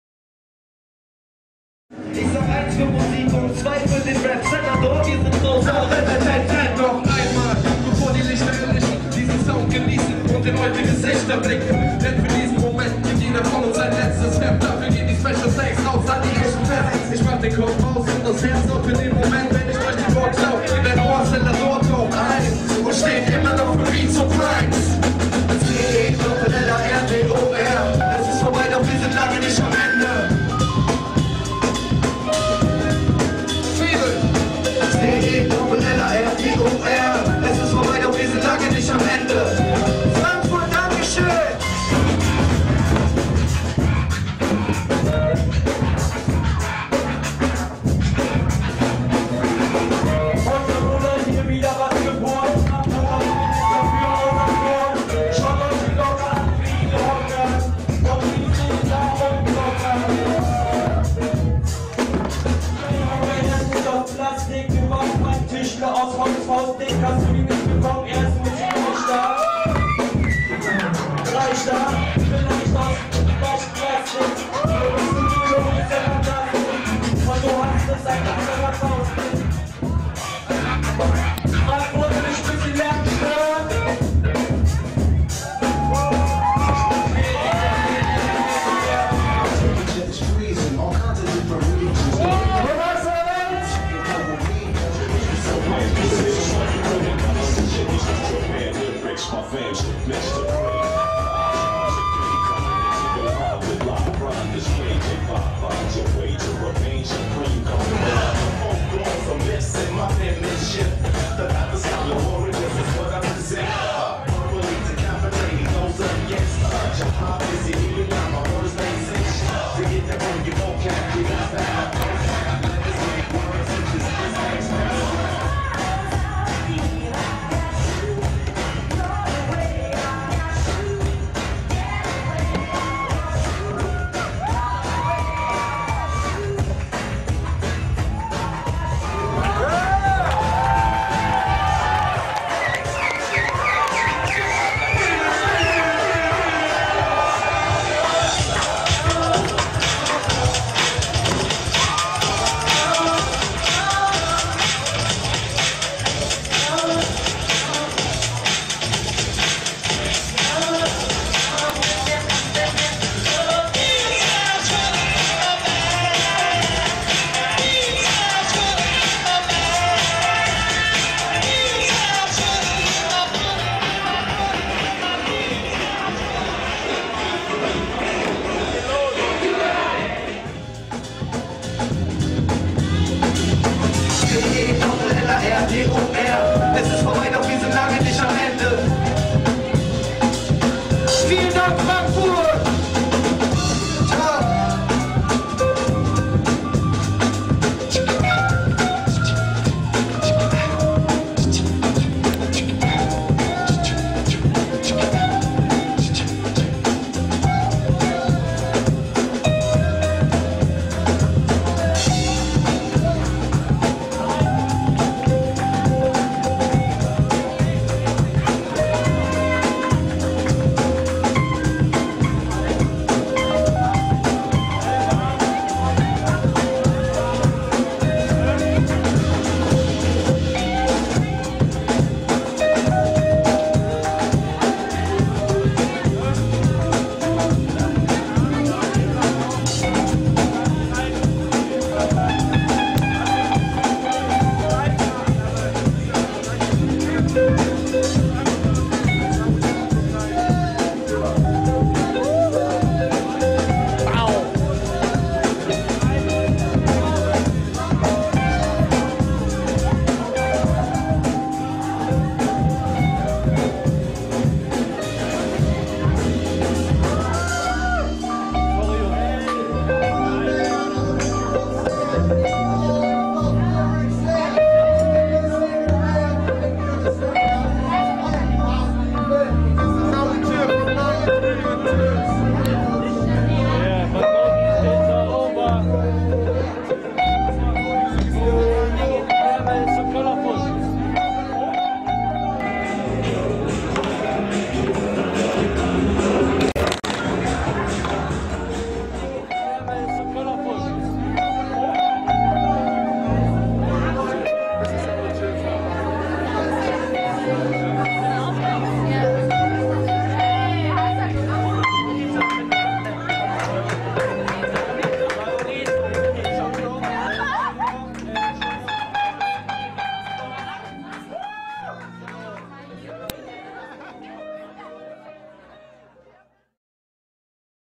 Frankfurter Turntable Hockern 2009
Frankfurter Turntable Hockern / Hock and Dance to the Beats
am 11. September 2009 im O25 (Ostparkstraße 25, 60385 Frankfurt/Main)